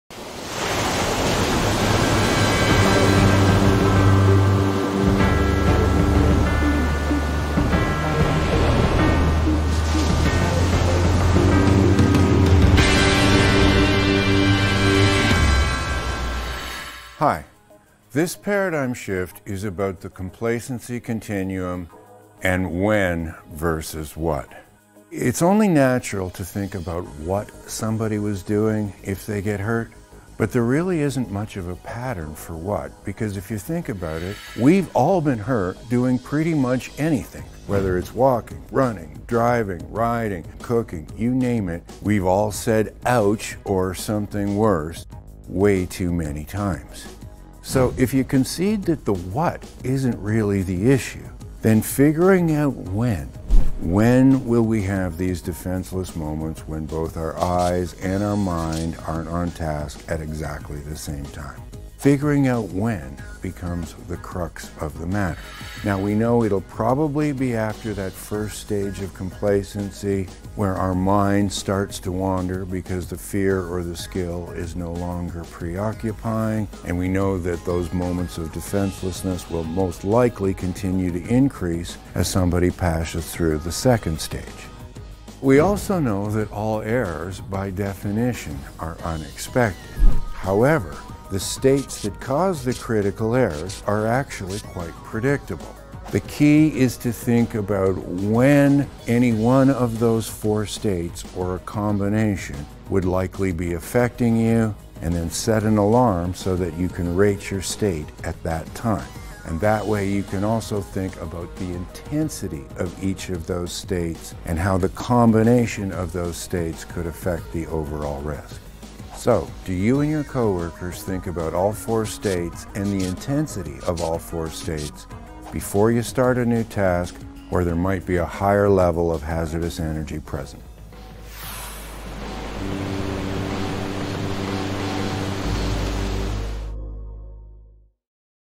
Paradigm Shifts by Larry Wilson | #8 - The Complacency Continuum and “When” vs. “What”
Knowing what actually causes the majority of serious injuries and fatalities is a good start, certainly much better than guessing or assuming that it’s due to a lack of management commitment. However, in order to prevent serious incidents and fatalities we need to know “when”. When will be most likely to make a critical error? This article explores the concept of Anticipating Error™ and provides practical tools for helping you and your employees to Rate Your State™ when in a situation where there is a high risk for making one or more critical errors that could cause a serious injury or expensive mistake.